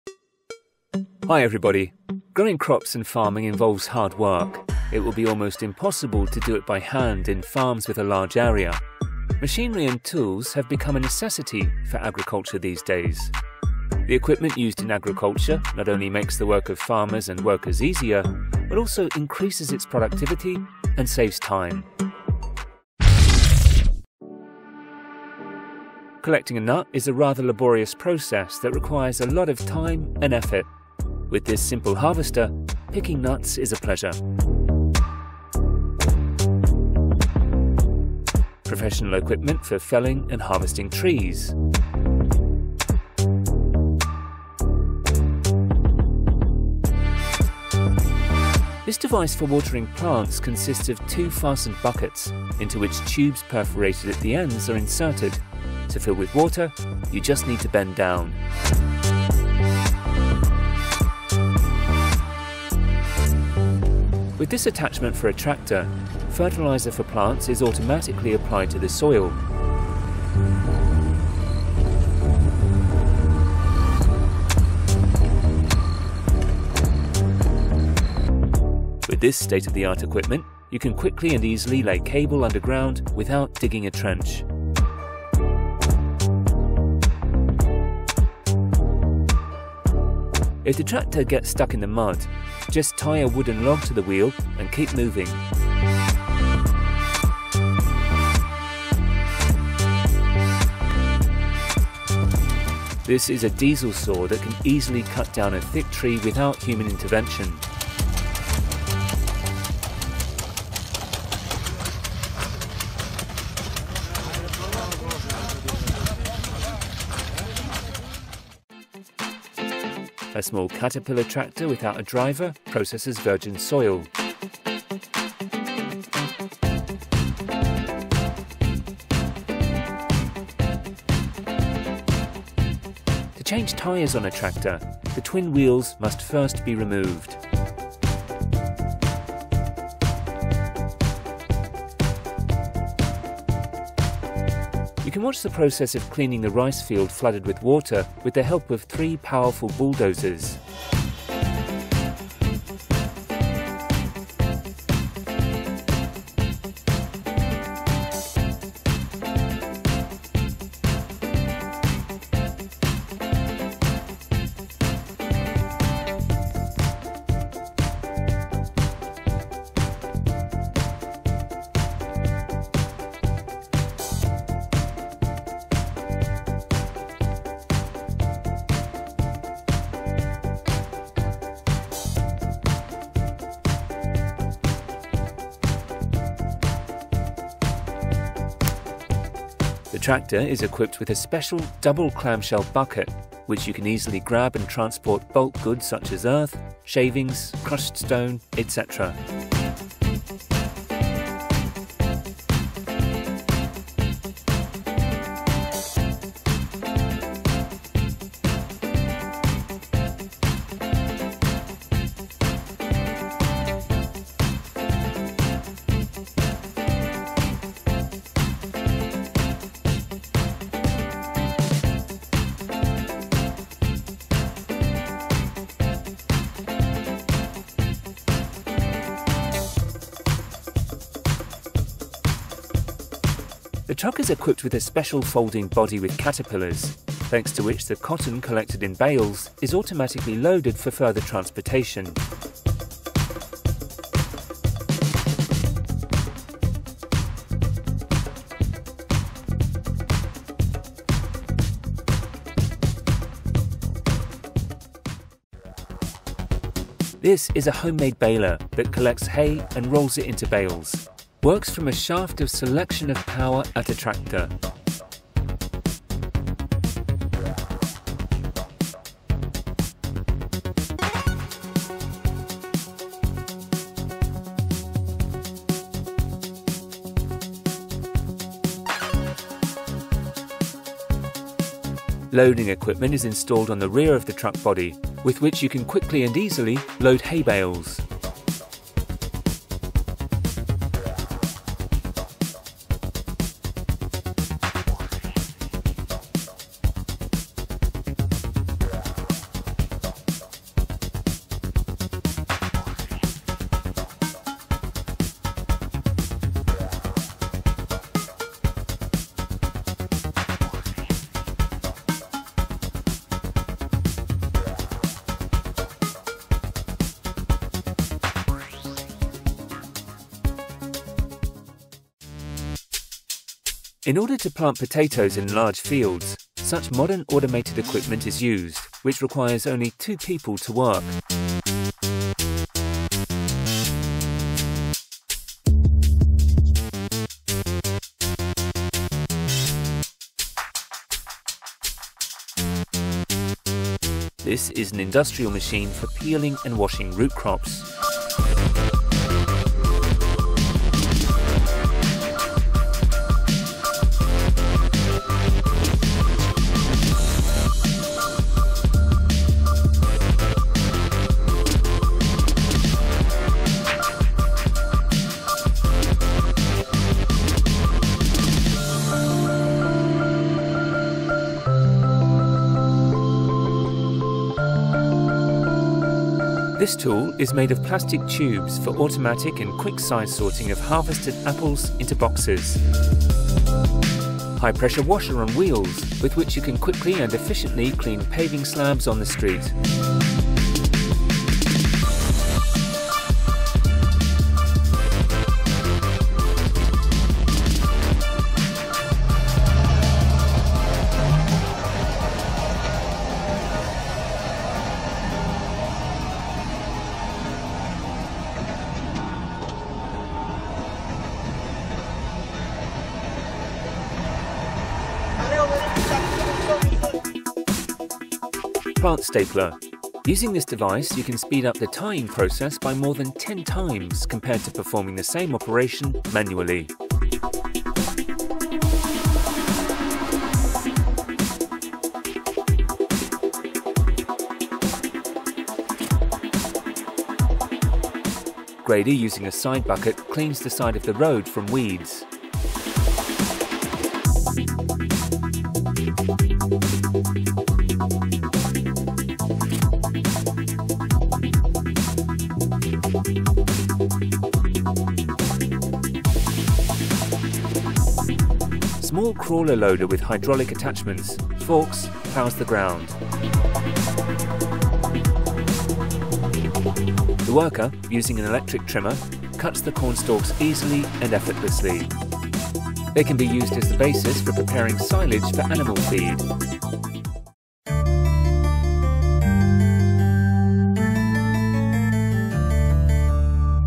Agricultural machinery and tools of another level
Growing crops and farming involves hard work, it would be almost impossible to do it by hand in farms with a large area. Machinery and tools have become a necessity for agriculture these days. The equipment used in agriculture not only make